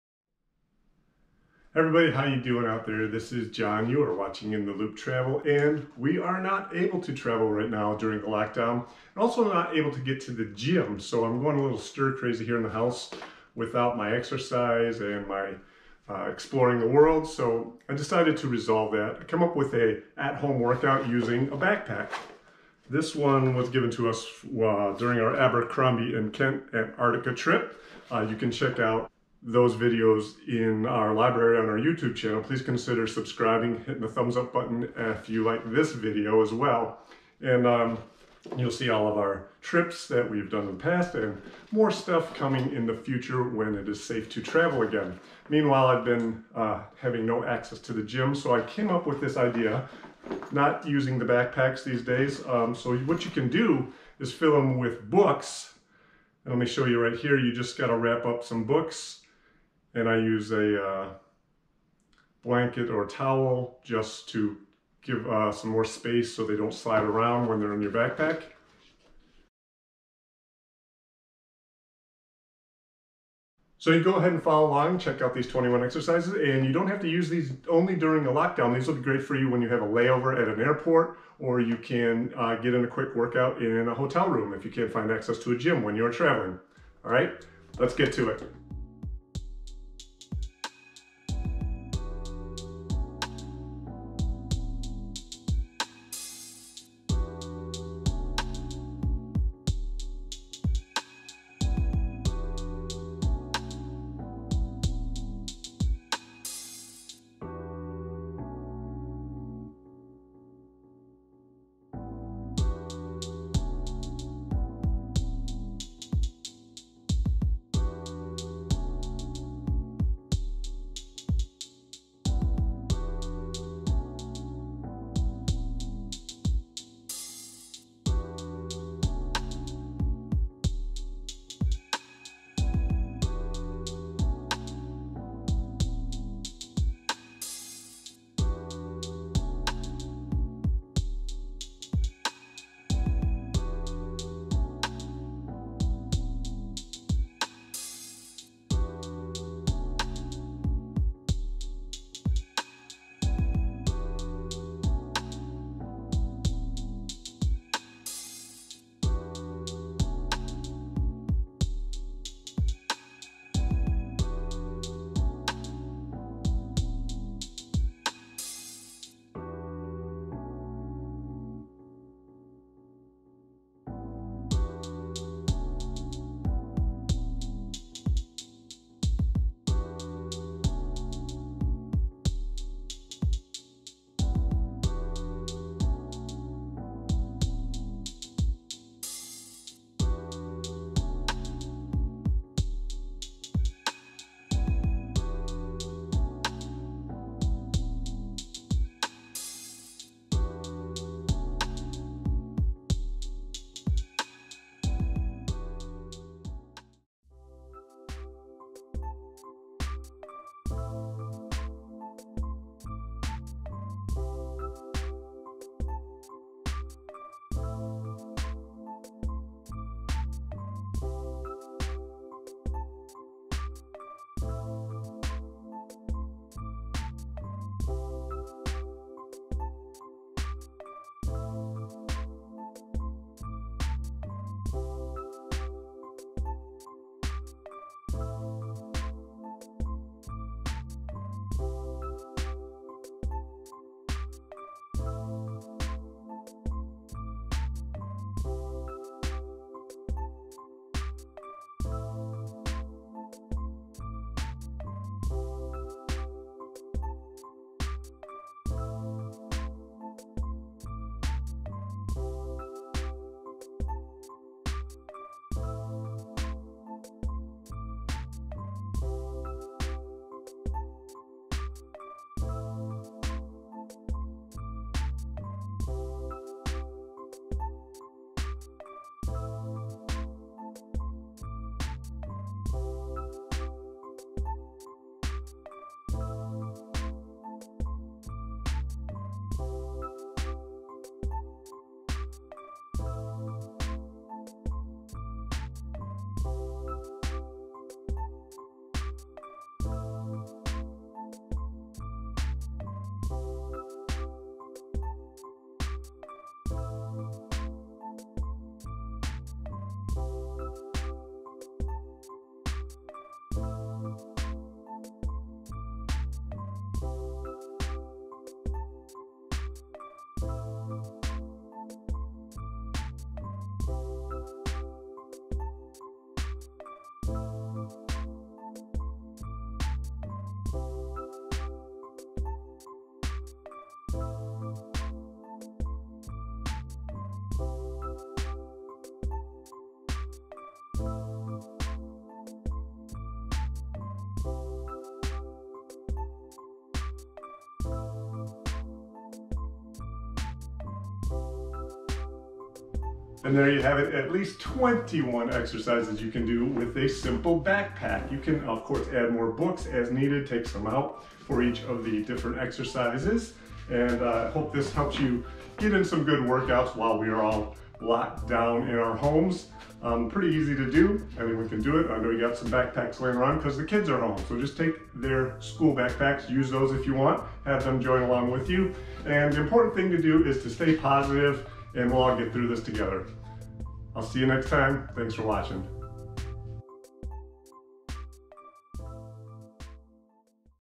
21 Backpack Exercises: Great for Travel (or a Lockdown)
You can use a simple travel backpack to get a good workout at home or during your travels. These backpack exercises work great when you can't get to a gym. You can use do these moves at home, on a ship, in a hotel room or at the airport.